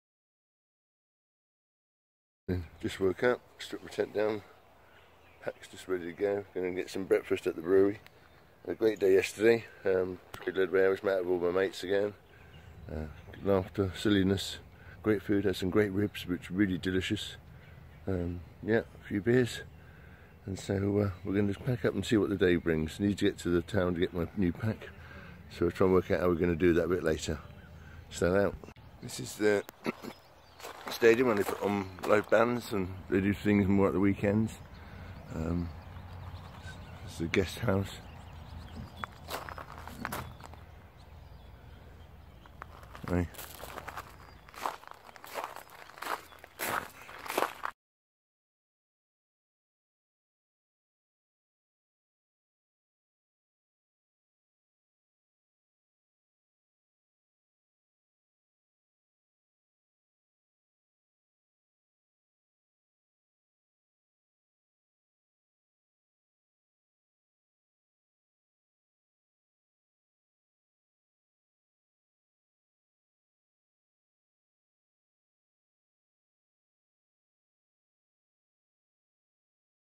Day 65 Heading to Waynesbroro Appalachian Trail 2018
Snailtrainer’s Appalachian Trail thru hike 2018
Heading to Waynesboro